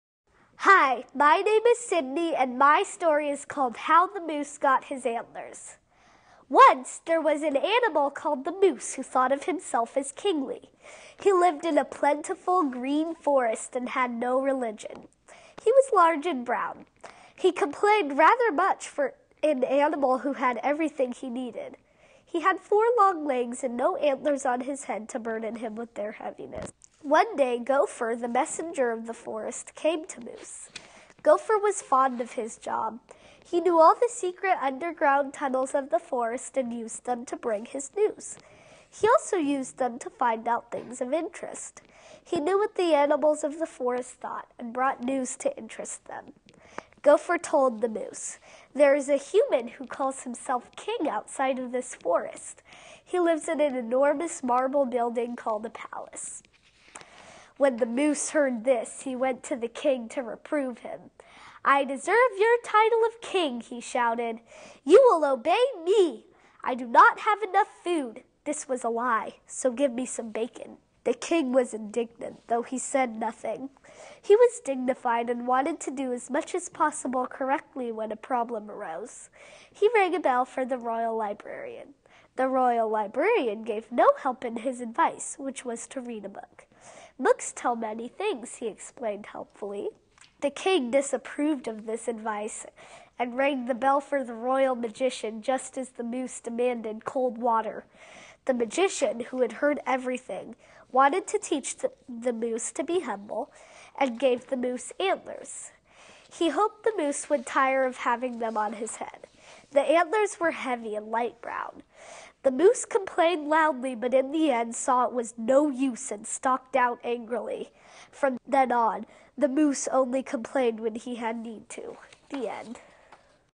2nd Place, 2nd Grade - 2014 Young Authors (K, 1st, 2nd Grade)
Sydney -- 2nd Place, 2nd Grade winner of the 2014 KPTS Young Authors Contest, reads "How the Moose Got His Antlers" and features creative hand-drawn illustrations!